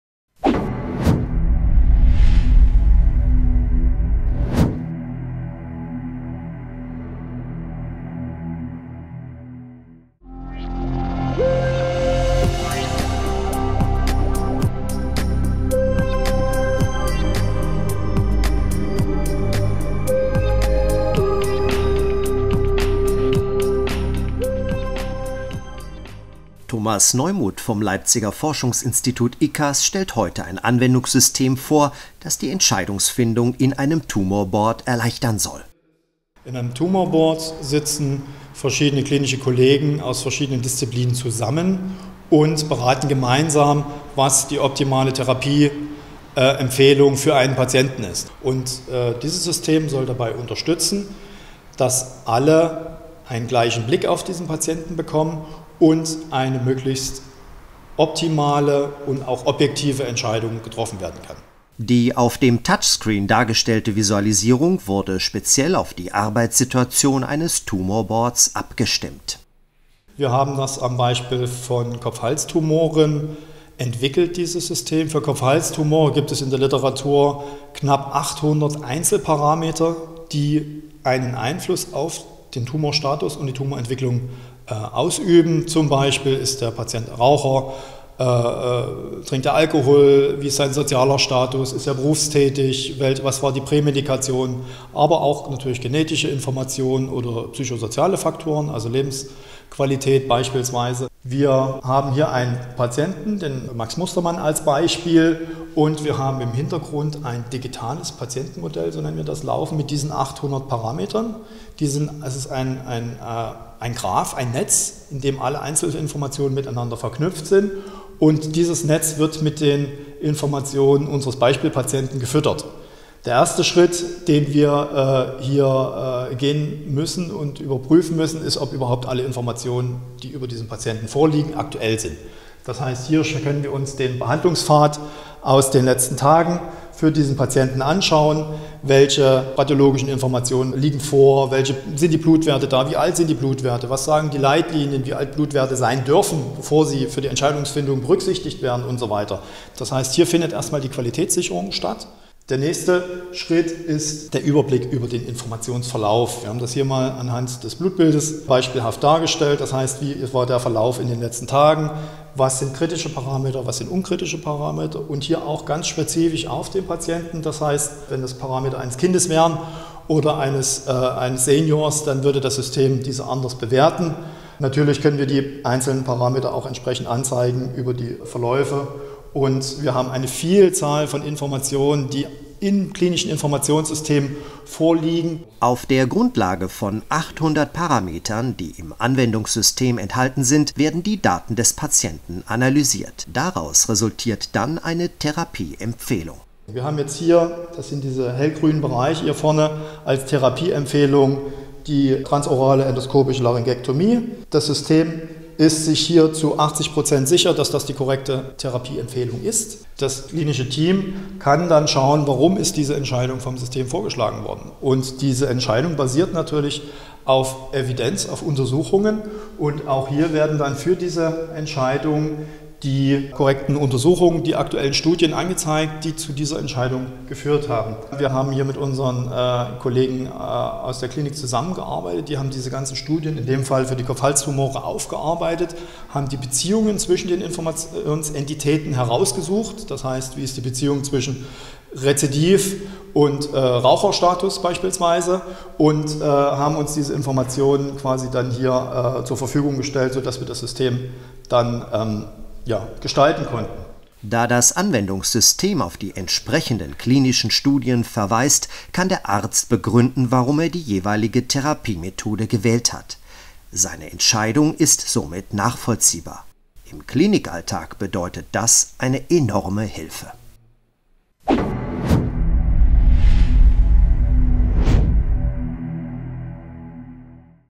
Hightech aus Leipzig - Computergestützte Medizin im OP
Teil 5: Entscheidungsunterstützungssystem für Tumorboards

Das Innovationszentrum für Computerassistierte Chirurgie (ICCAS) mit Sitz in Leipzig ist Vorreiter bei der Entwicklung computergestützter, Technologien, intelligenter Assistenzsysteme und bildgestützter Interventionen. Es bildet die Schnittstelle zwischen klinischen Anforderungen und wirtschaftlicher Umsetzung wegweisender biomedizinischer Technologien nach Medizinproduktegesetz. Ziel ist der Verbesserung von Therapiemethoden und Arbeitsabläufen zur Erhöhung der Patientensicherheit und der gesundheitsökonomischen Effizienz.